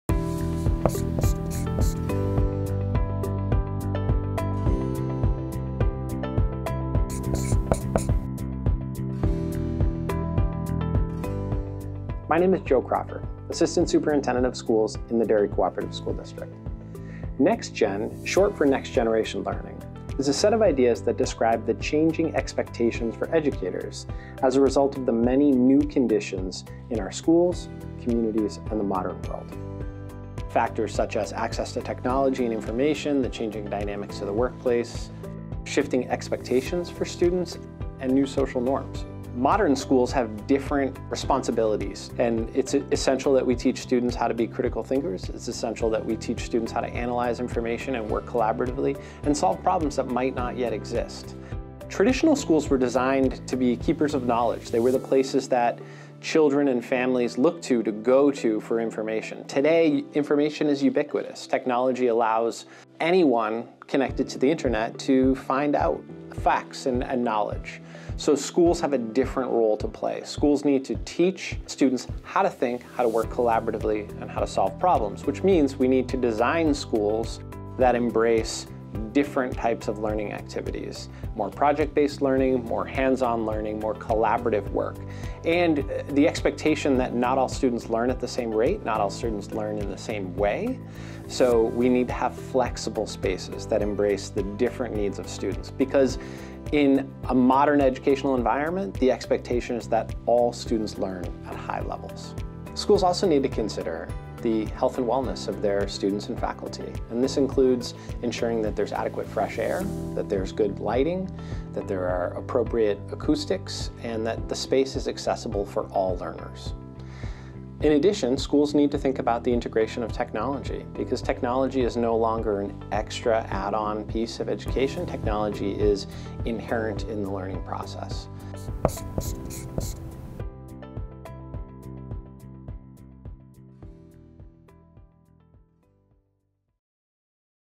DCSD Q&A: NextGen Learning
What do we mean by NextGen or 21st Century Learning?